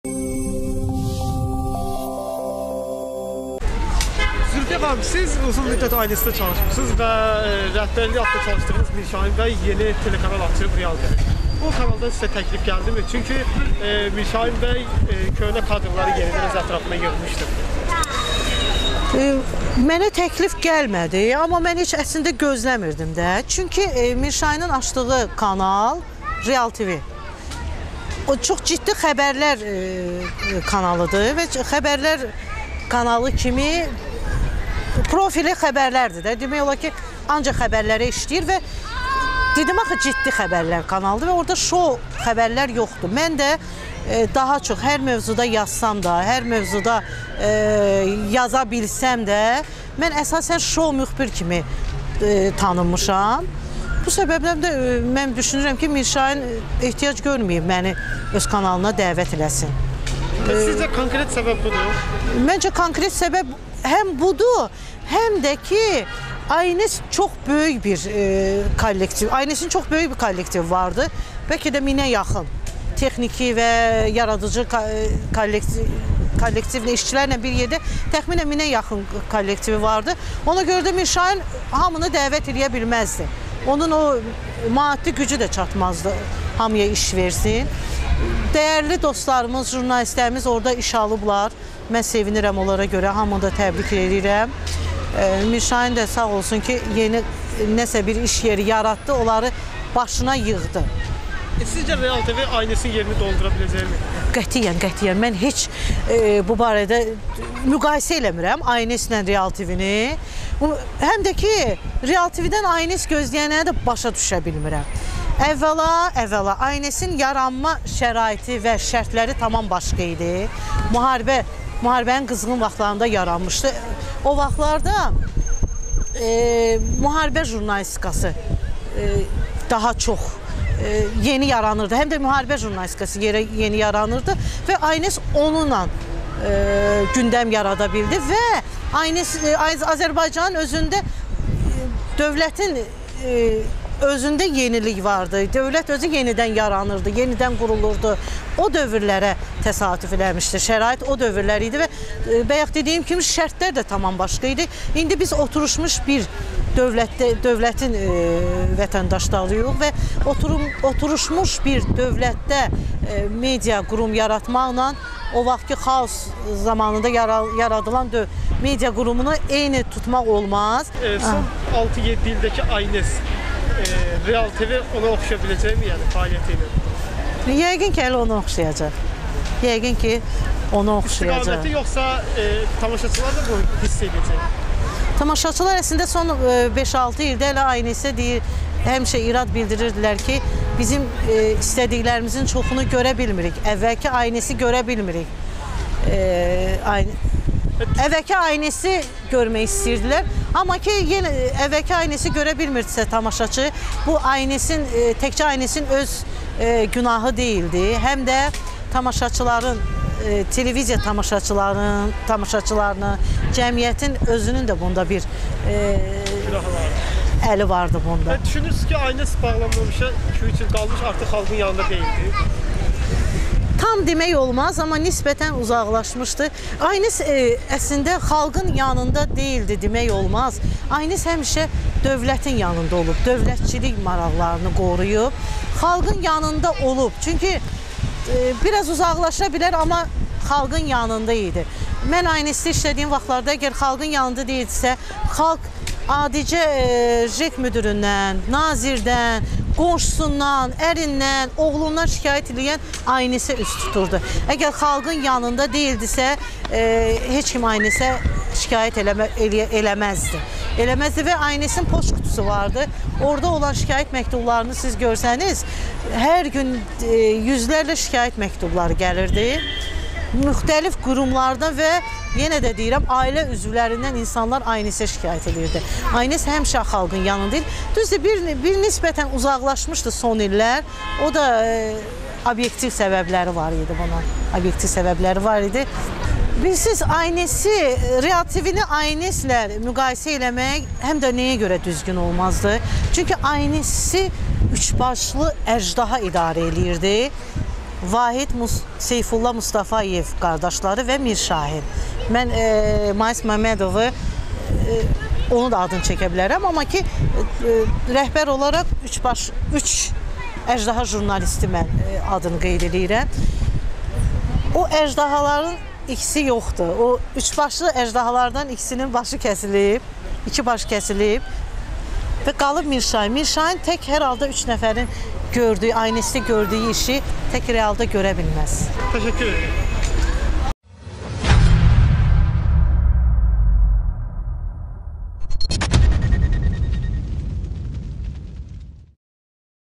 "ANS-i 3 başlı əjdaha idarə edirdi, "Real"-ı isə təkcə Mirşahin"-Keçmiş ANS işçisi
ANS telekanalının keçmiş işçisi Zülfiyyə Qurbanova:  "Real Tv" ciddi xəbər kanalıdır.Mən isə əsasən şou müxbir kimi tanınmışam. Yəqin ona görə də M.Ağayev məni dəvət etməyib.
"Real Tv" qətiyyən ANS-in yerini doldura bilməz.ANS müharibə şəraitində yaranmışdı və ANS-i 3 başlı əjdaha idarə edirdi ancaq "Real"-ı təkcə Mirşahin idarə edir. İnsanlar son 5-6 ildə deyirdilər ki, ANS onların istədikləri xəbərləri vermir yəni, əvvəlki ANS-i görə bilmirdilər. Burda isə təkcə ANS yox, tamaşaçlılar da günahkar idi. ANS əvvəllər daim xalqın yanında olub, son illərdə isə bir az uzaqlaşmışdı".